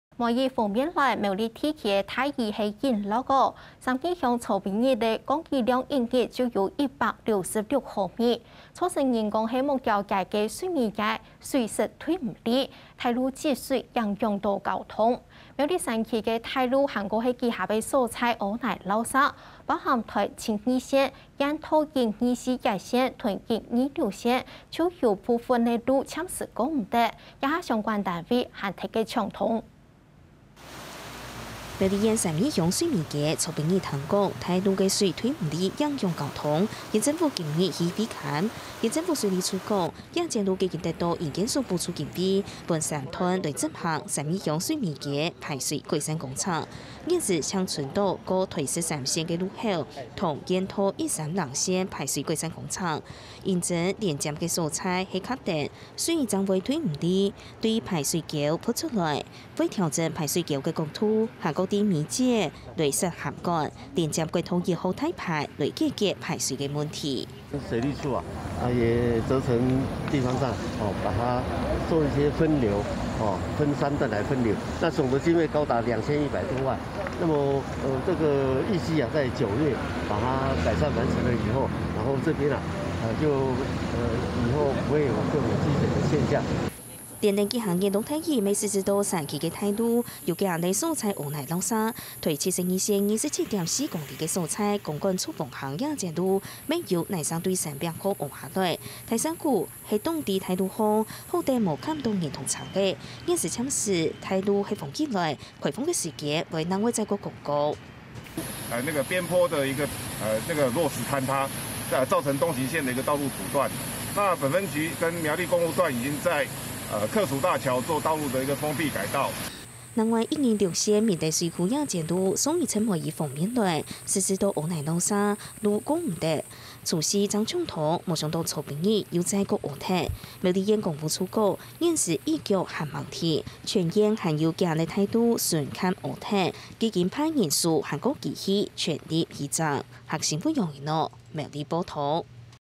大雨釀災 苗栗三義水美街淹水.山區傳坍方【客家新聞20220608】
梅雨鋒面來襲，苗栗地區大雨不斷，三義鄉7日降雨量單日高達166毫米，使得木雕勝地水美街，也一度傳出排水宣洩不及，馬路積水影響交通，而苗栗山區道路，更是多處出現落石、坍方，包括臺72線、縣道124乙線，以及126線，都有部分路段暫時封閉，目前相關單位正進行搶修。